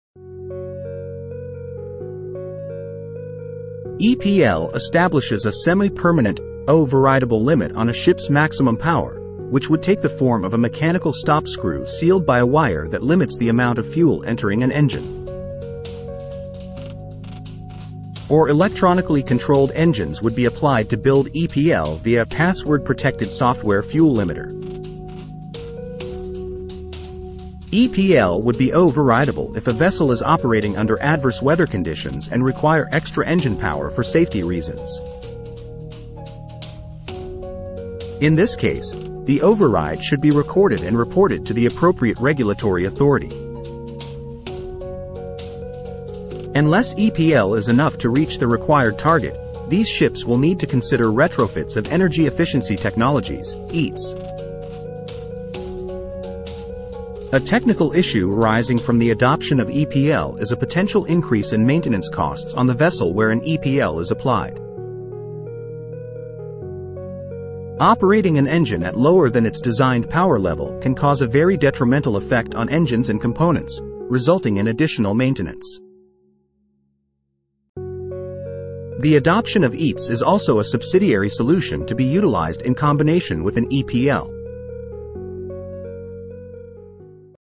EPL the easiest way to meet eexi requirements
The alternative compliance options available for vessels to meet their target EEXI values include a reduction in propulsive power know as an Engine Power Limitation (EPL), retrofitting of Energy Efficiency Technologies (EETs), or switching to a lower carbon fuel. EPL is likely to be the easiest way for older vessels to meet EEXI requirements since it requires minimal changes to the vessel and doesn’t alter the underlying performance of the engine.